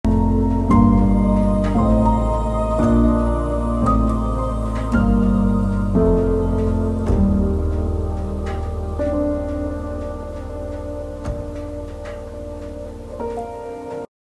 SALE SALE SALE!
Elevate your space effortlessly with our reliable diffuser, creating a continuous, inviting scent that lasts for weeks. Experience the perfect blend of freshness, elegance, and a touch of luxury with our flower reed diffuser.